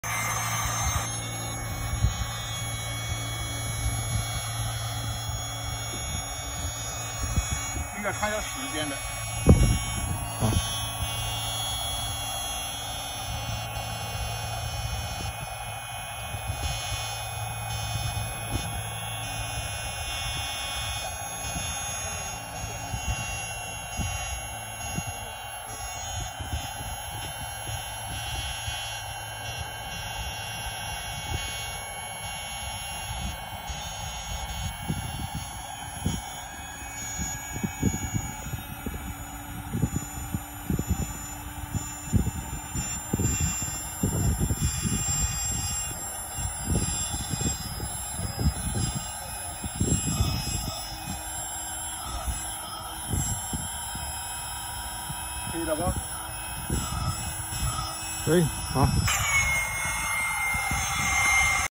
Rechargeable quick pump
Rechargeable quick pump: every full charge, it could work 30-40 minutes. It’s 1 minute to charge such half kayak.